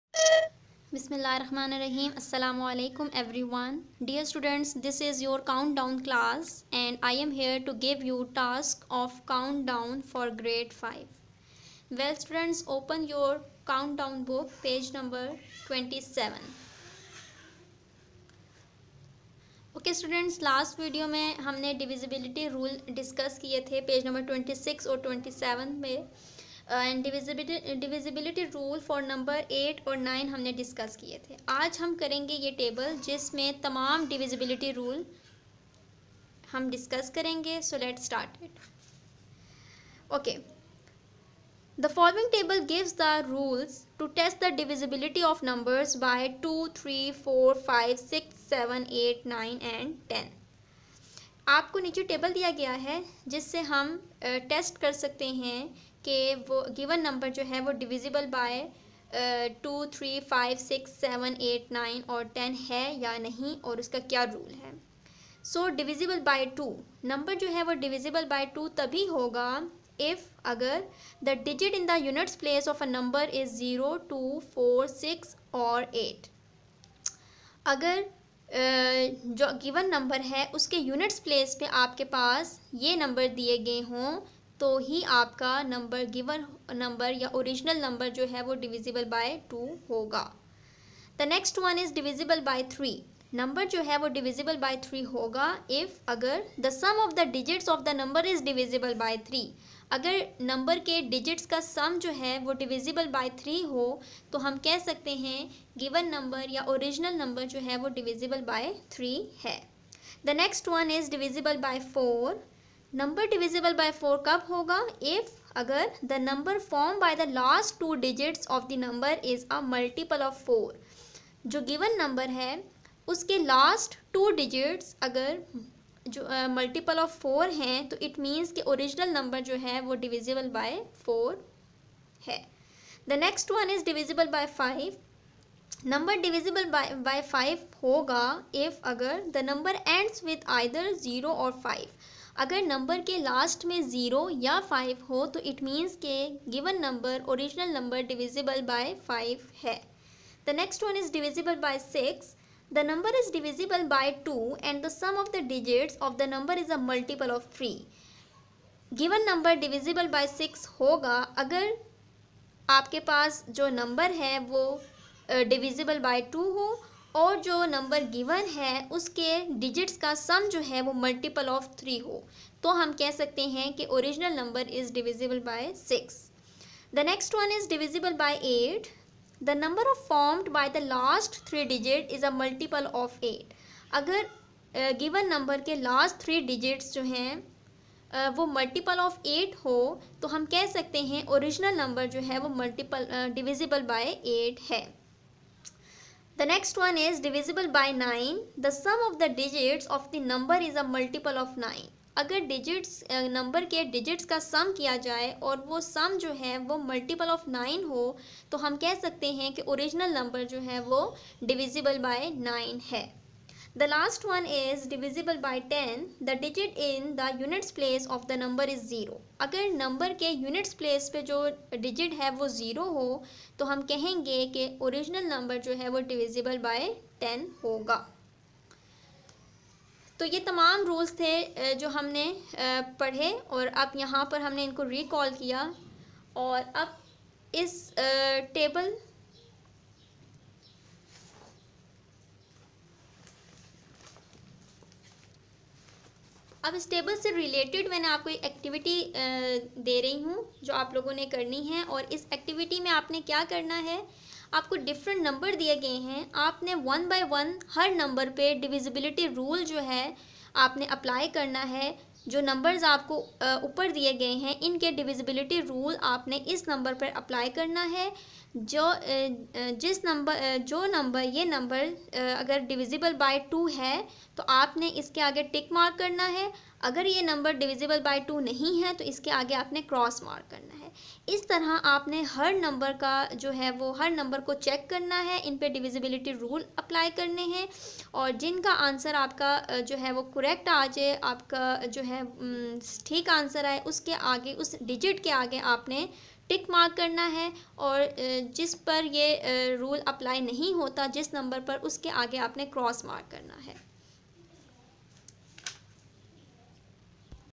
COUNTDOWN~ CLASS 5~ pg#27~ Divisibility Rule for the numbers 2,3,4,5,6,8,9,10.
In this video you will learn about divisibility rule for the numbers 2,3,4,5,6,8,9,10.
# divisibility rule